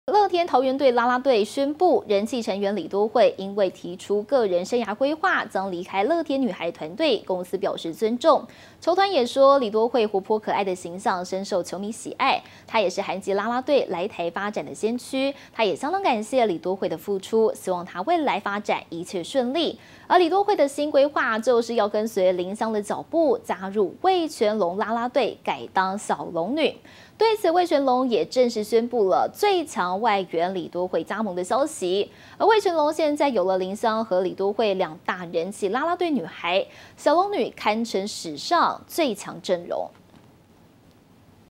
Thank you！ 李多慧因「生涯規劃」離開樂天女孩【最新快訊】
樂天桃猿隊 啦啦隊 正式宣布，人氣成員 李多慧，因為 提出個人生涯規劃，將離開 樂天女孩團隊，公司表示 尊重。球團 表示，李多慧 活潑可愛的形象，深受球迷喜愛，她也是 韓籍啦啦隊 來台發展的先驅；樂天也相當感謝 李多慧的付出，希望她未來發展一切順利。而 李多慧的新規劃，就是跟隨 林襄腳步，加入 味全龍啦啦隊，改當小龍女；對此，味全龍也宣布 最強外援李多慧 正式加盟！而味全龍，現在有了，林襄和李多慧 2大人氣啦啦隊女孩，「小龍女」堪稱，史上 最強陣容。